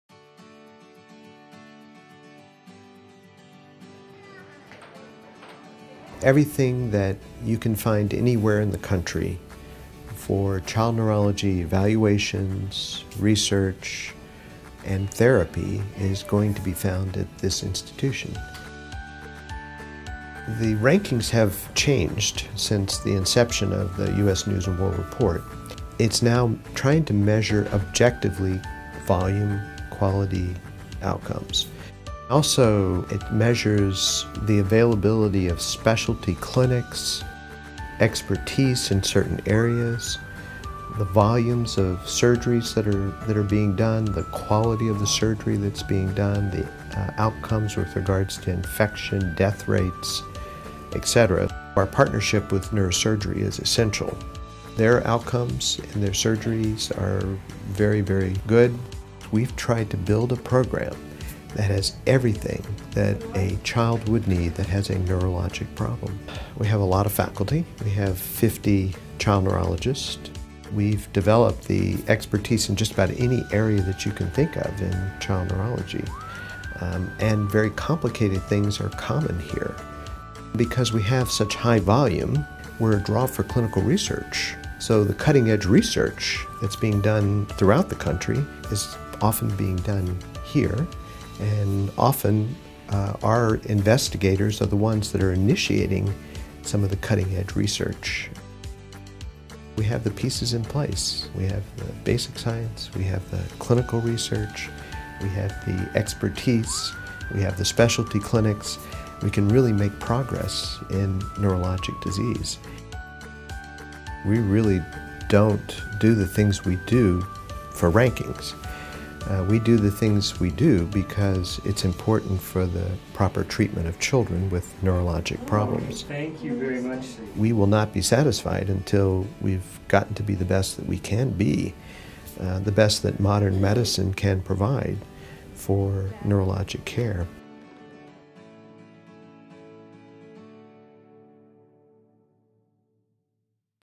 Behind the U.S.News & World Report Rankings: Neuroscience Center
Neurology and Neurosurgery at Texas Children’s Hospital is ranked #4 by U.S.News & World Report. In this video, let the Chief of Neurology, Dr. Gary Clark, explain what this ranking means for the measurement of volume, quality and outcomes and how this relates to the care of our patients. To learn more about the Neuroscience Center at Texas Children’s Hospital,